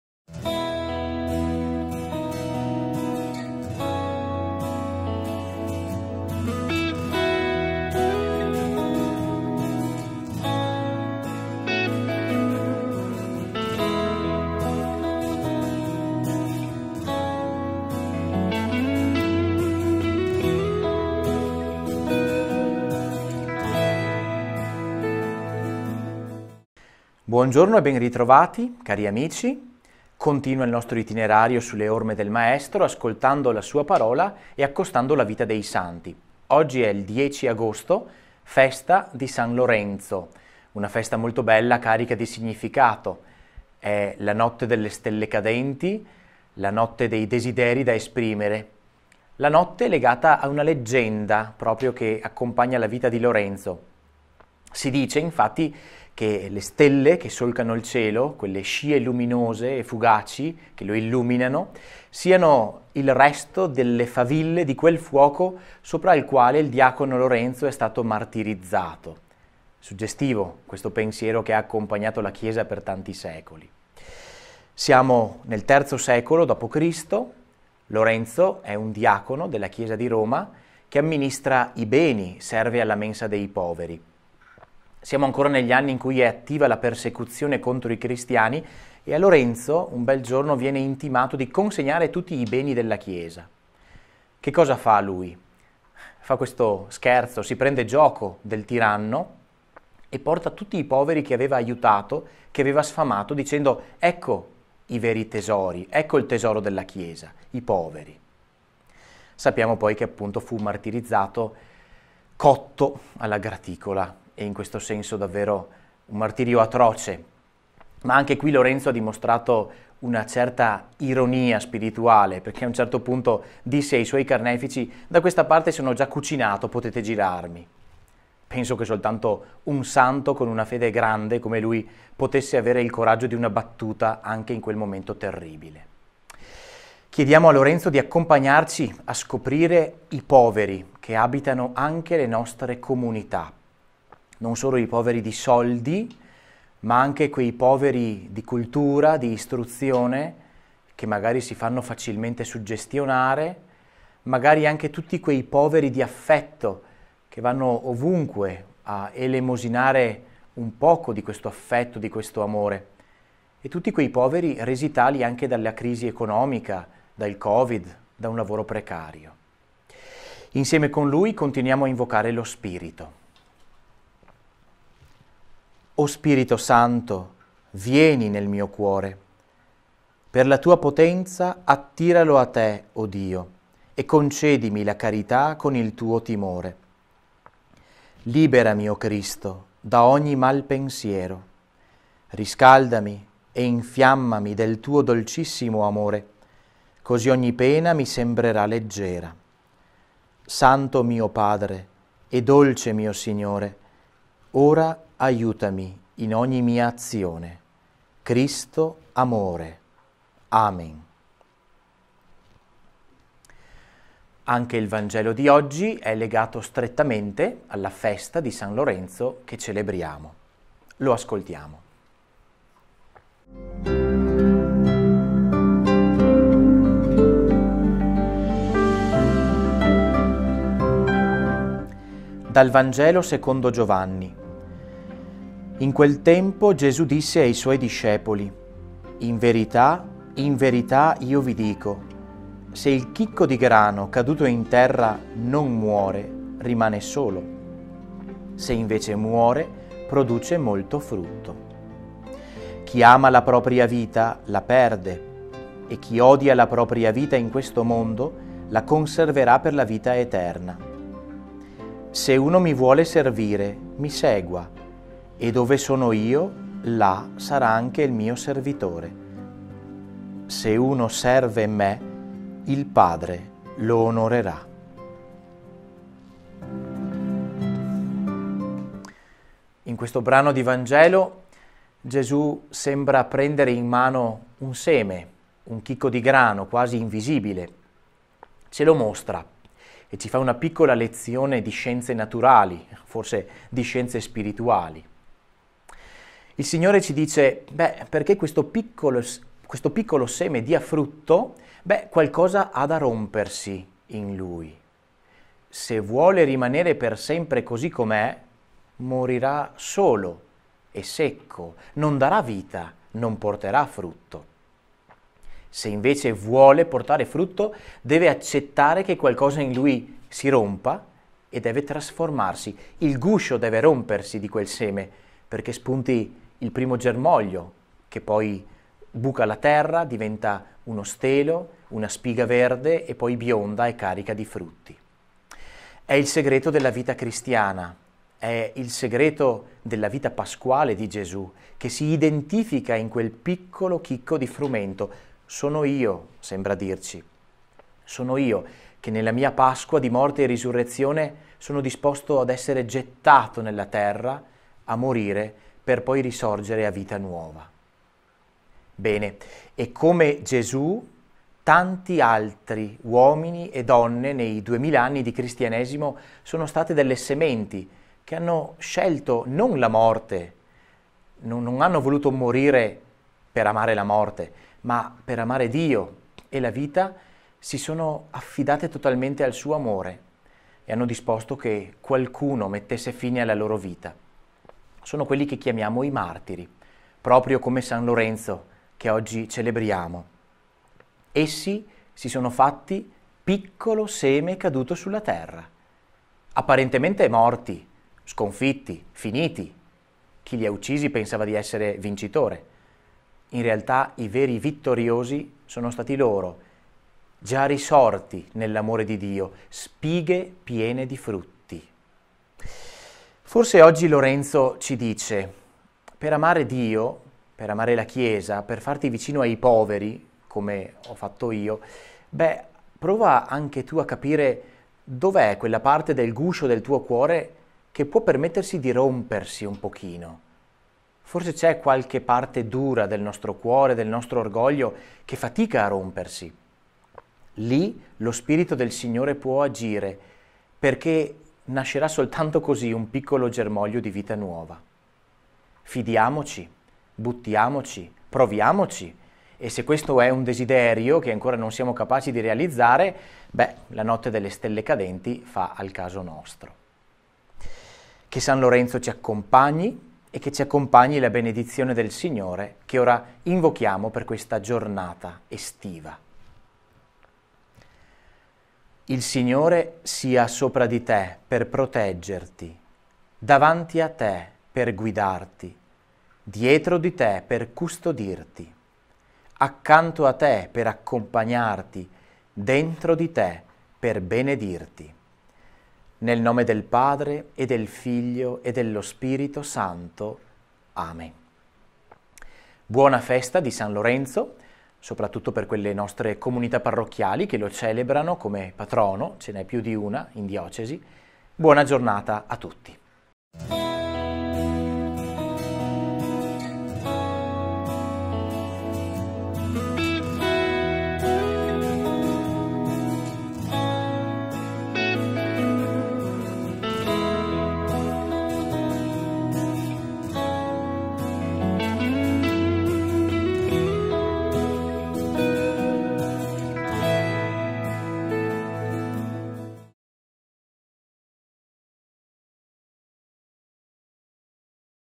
Sulle orme del Maestro - Martedì 10 agosto 2021
Riflessione sul vangelo del giorno a cura di don Fabio Mantese. Regia Federico Campodall’Orto. Una produzione La Tenda Multimedia